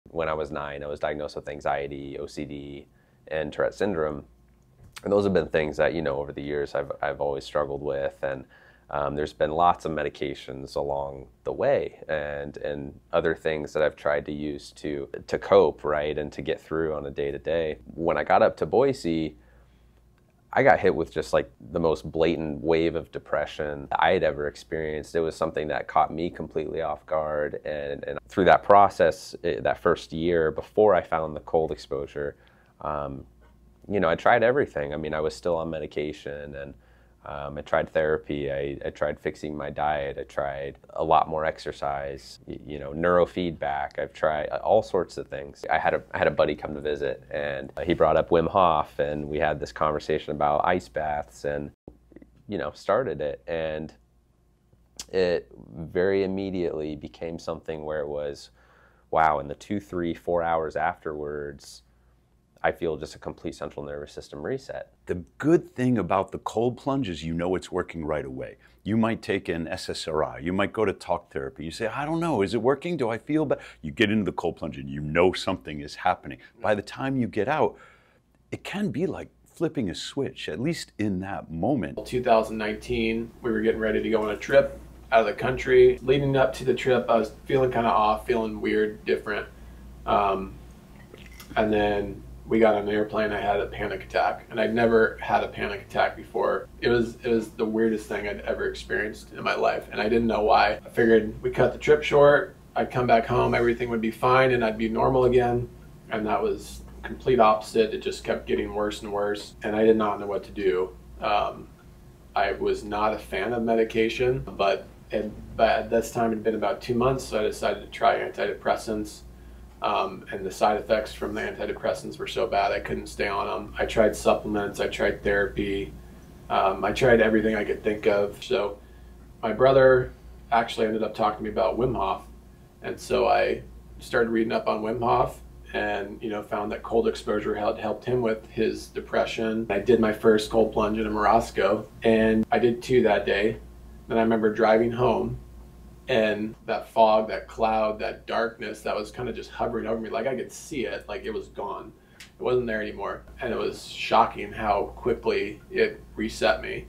Ice Baths For Depression | Morozko Health Freedom
Cold plunge therapy lifts mood, boosts metabolism, and produces neuroprotective factors that support brain health. That may explain several scientifically documented studies that show cold water immersion is effective for resolving major depression, even where therapy and medications fail.

At the Cold Plunge Research Planning Workshop in Phoenix AZ, Cold Plunge Cam and Matt from Desert Plunge shared their testimony with Prof. Seager and Edgars Treimanis (not pictured) about overcoming mental health challenges. Both men discovered that cold water could do for them what no medication ever could, and that they've either discontinued or tapered off their meds as a result.

Do you know someone who has seemingly tried everything and feels that nothing works?

Perhaps the stories these men are telling about themselves will inspire others to give the icebath a try.